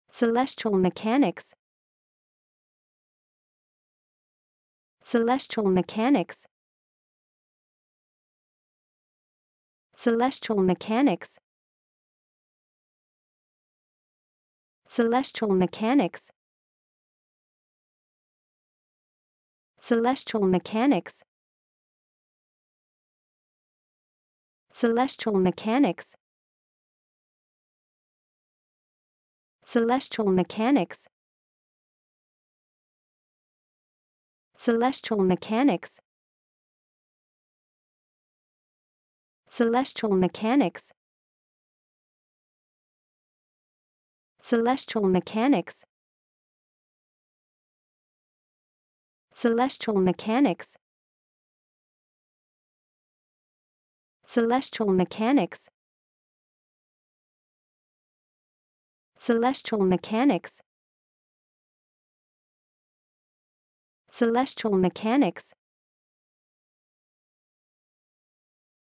Como dizer 'mecânica celeste' em ingles?
Como dizer mecânica celeste em Ingles? Como se pronuncia celestial mechanics? How to pronounce celestial mechanics?